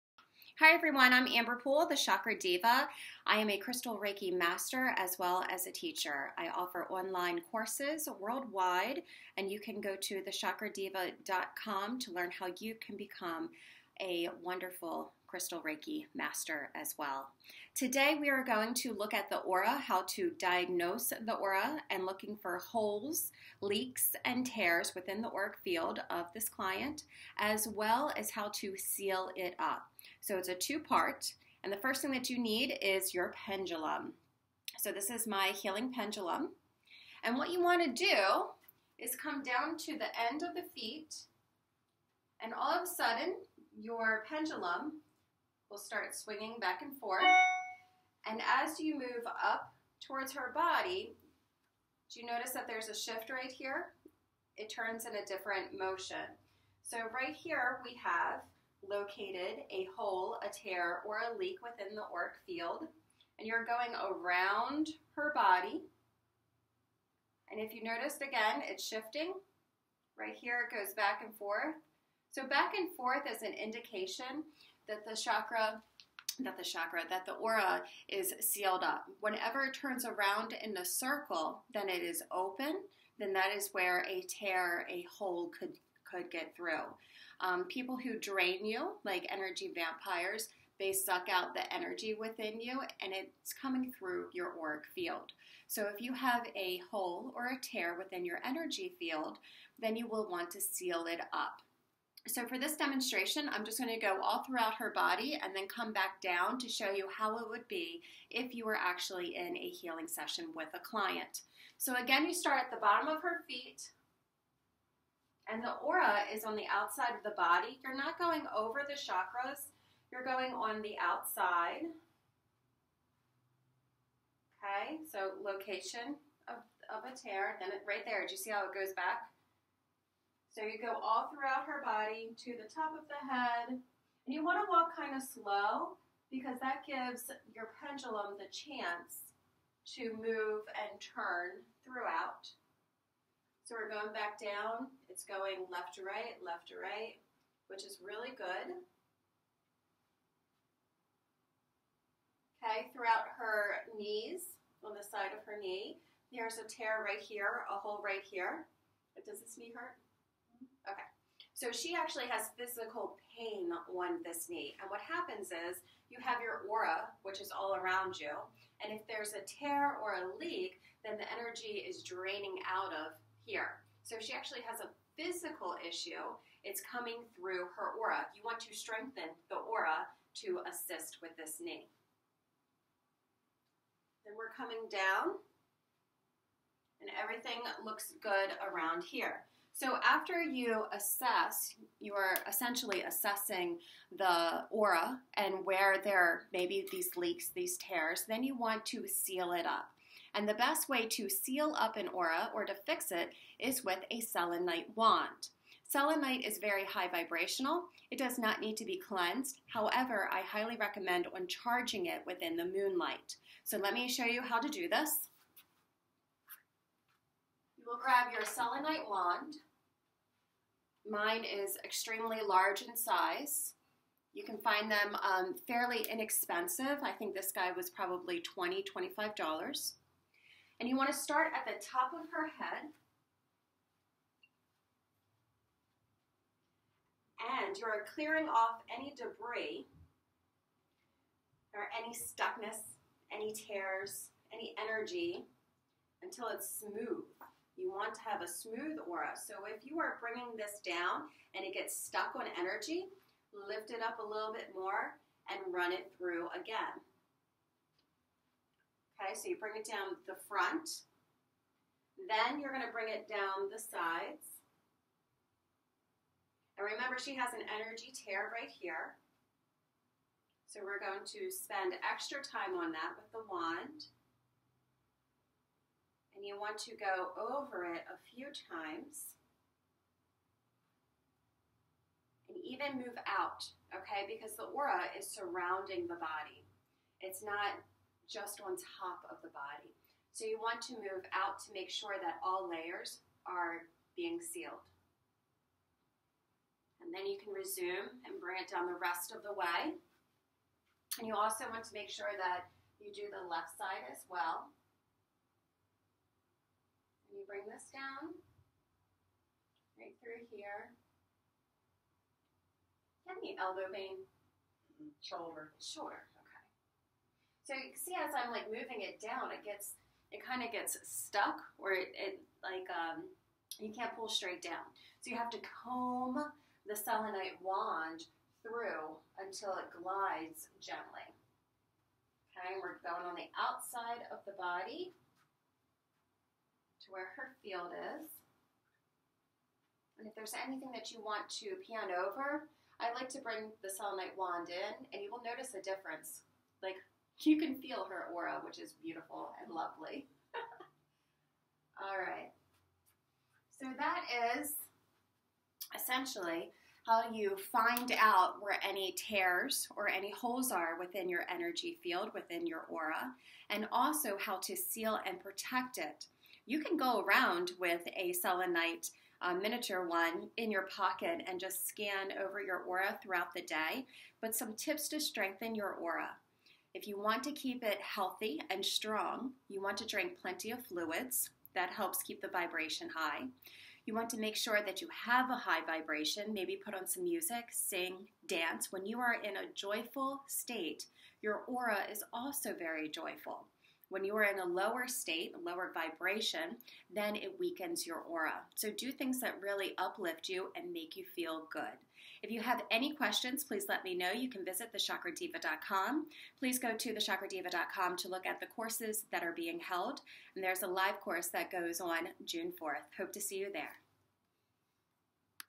Detecting Holes and Leaks in the Aura
How to Find Aura Imbalances and Seal Them